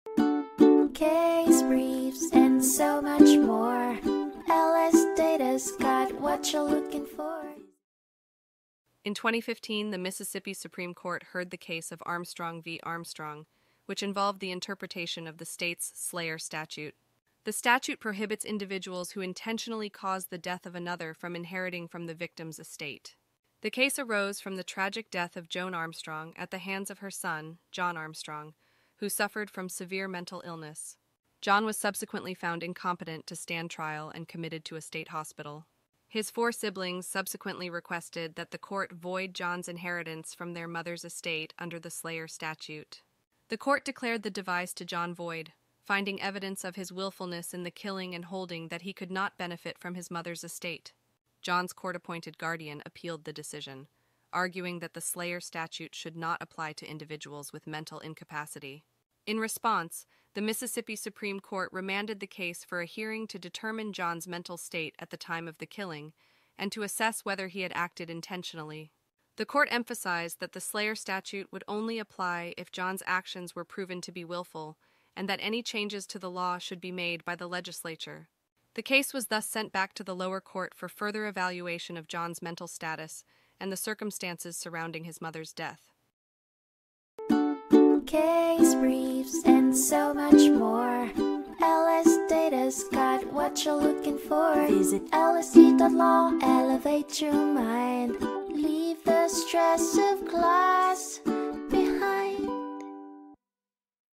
Armstrong v. Armstrong (2015) Overview | LSData Case Brief Video Summary
John Armstrong killed his mother and was found incompetent to stand trial. His siblings wanted his inheritance from their mother's estate voided under the Slayer Statute that prohibits anyone who intentionally causes the death of another from inheriting from the victim. The court found evidence of John's willfulness in his mother's killing and declared the devise void under the Slayer Statute. John's brother appealed, and the court remanded the case for a hearing to determine John's mental status at the time of the killing and whether he acted intentionally.

Armstrong v. Armstrong (2015)
Mississippi Supreme Court
170 So. 3d 510